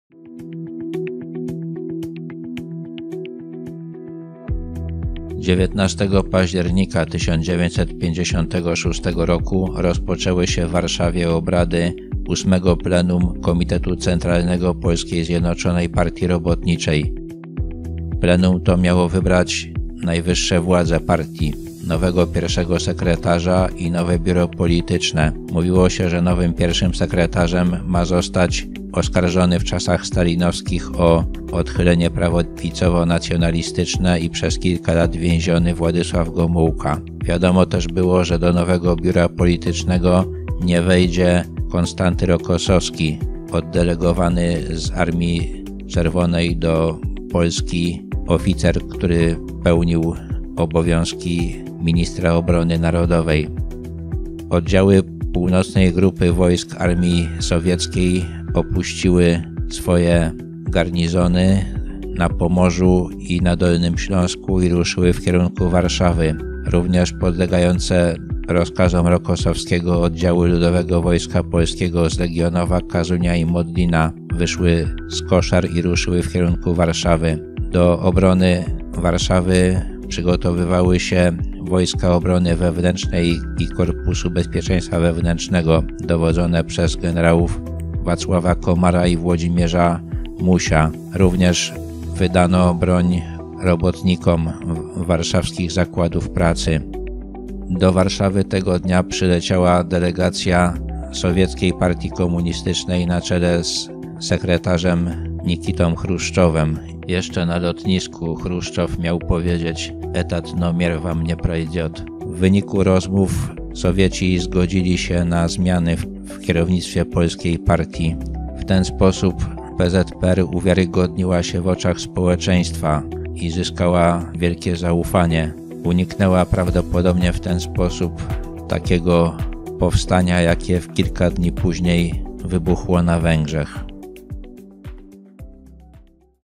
Kartka z kalendarza 19.10.2020 VIII Plenum KC PZPR
19 października 1956 roku rozpoczęło się VIII Plenum Komitetu Centralnego PZPR. Pomimo sowieckich nacisków wybrało ono do władz ludzi, których naród obdarzył pewnym zaufaniem.